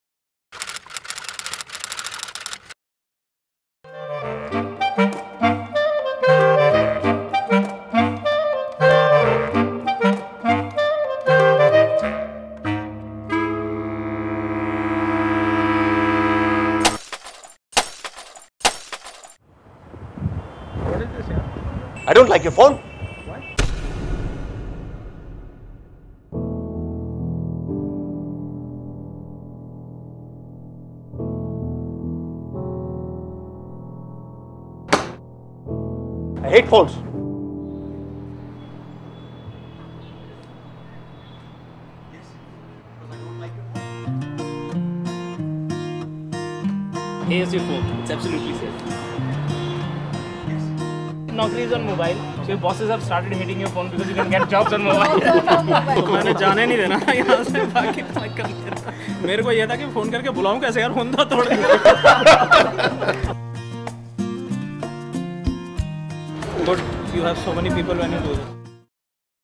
Angry boss smashing phone
Angry boss prowling around offices, smashing employee's phone - all because Naukri.com is on mobile!